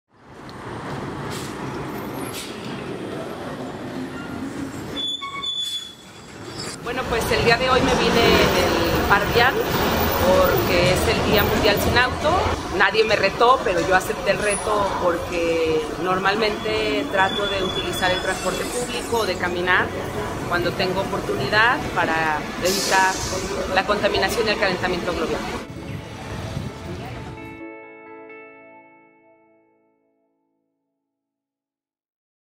Candelaria Ochoa
La regidora de Movimiento Ciudadano, Candelaria Ochoa utilizando el transporte público en el DiaMundialsinAutos